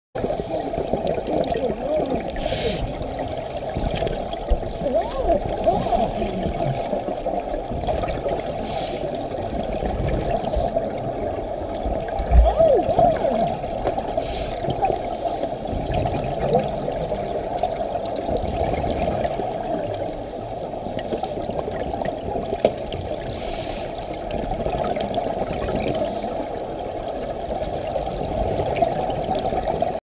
Shark punched repeatedly by the diver
Diver flies into rage repeatedly punching shark in front of horrified tourists